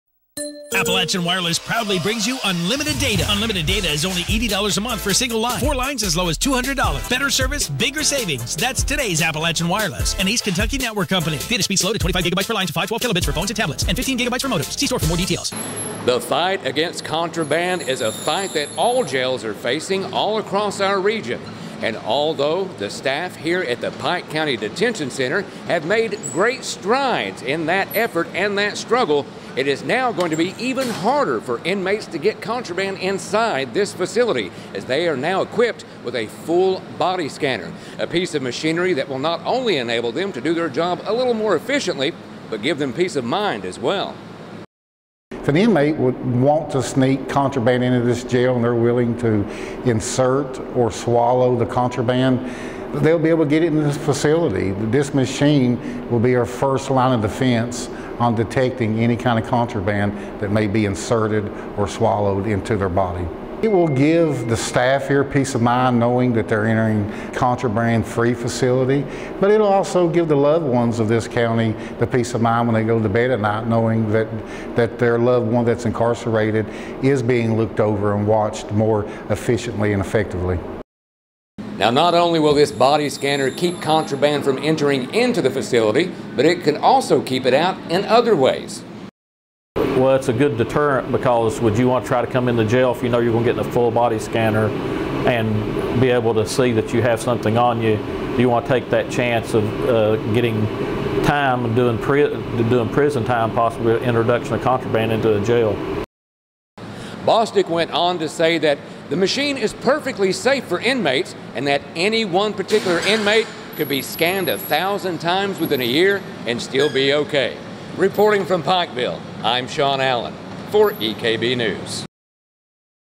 Jail now equipped with full body scanner
PIKE COUNTY, KY.-- A local jail is using a high-tech solution for stopping drugs and other prohibited items from coming in the door. EKB News Reporter Shawn Allen has the story